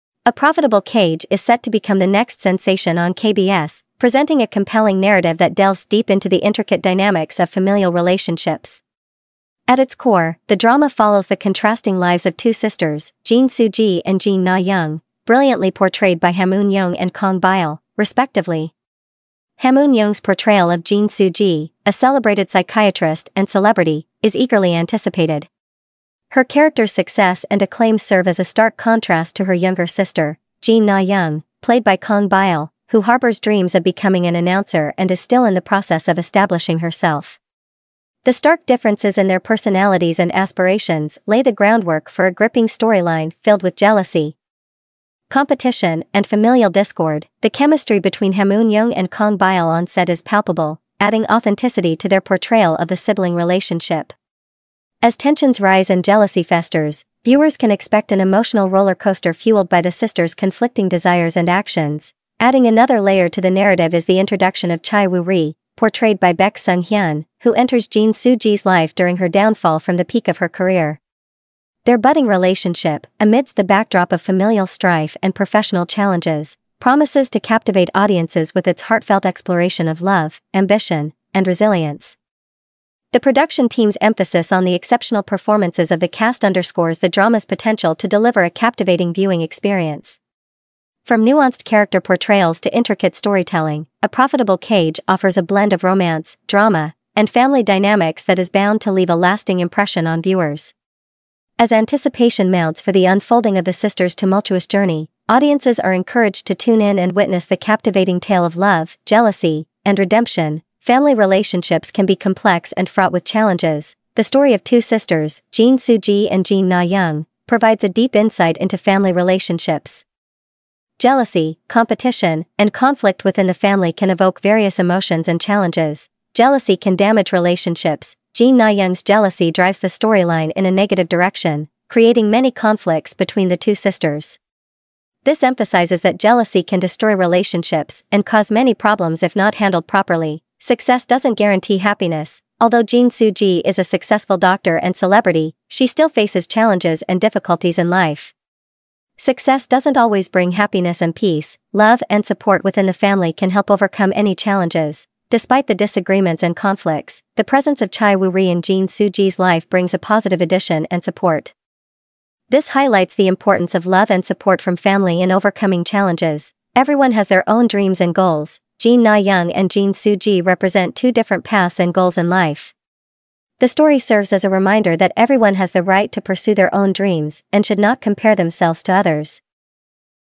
Fierce Family Feuds: Ham Eun Jung and Kang Byul's Intense Clash in 'A Profitable Cage' - ACNFM News
ACNFM - Asian Celebrities News: "A Profitable Cage" is set to become the next sensation on KBS, presenting a compelling narrative that delves deep into the intricate dynamics of familial relationships. At its core, the drama follows the contrasting lives of two sisters, Jin Soo Ji and Jin Na Young, brilliantly portrayed by Ham Eun Jung and Kang Byul, respectively.